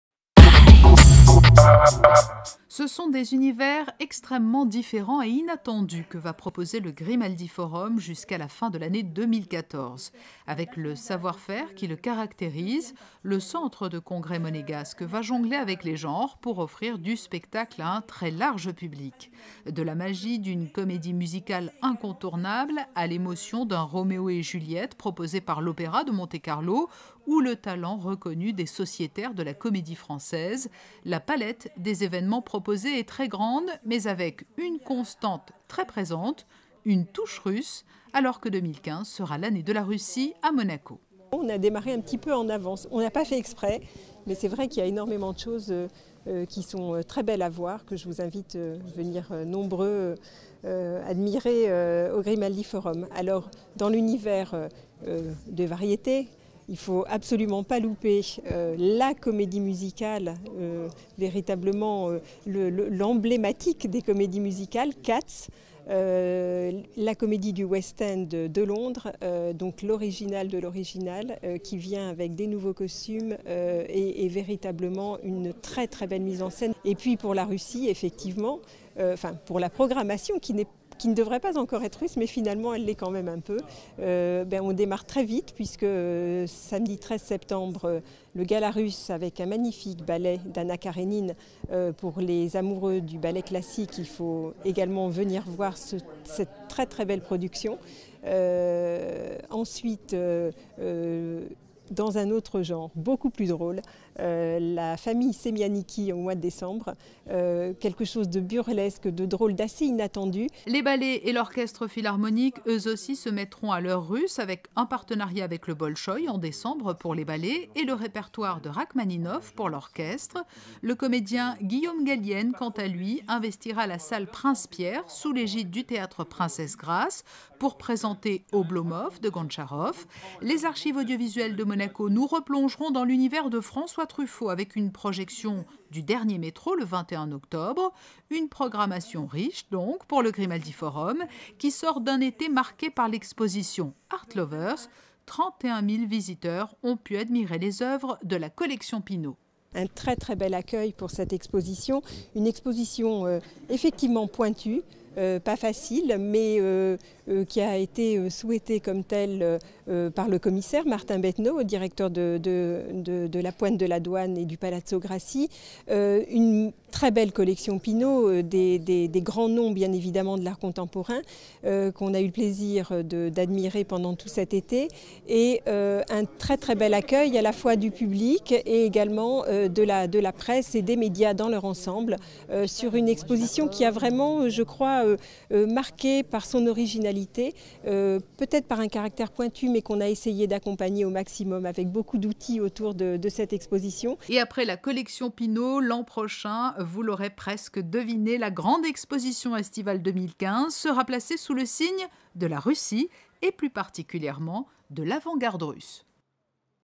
Le Grimaldi Forum de Monaco clôt 2014 en beauté
Des chats qui chantent, un Guillaume Gallienne paresseux, un hommage à François Truffaut, Rachmaninov superstar, le retour de Roméo et Juliette, des clowns russes tout sauf tristes et une Mégère Apprivoisée par un chorégraphe monégasque invité par le Bolchoï... Présentation à la presse de l'Agenda du Grimaldi Forum Monaco pour le second semestre 2014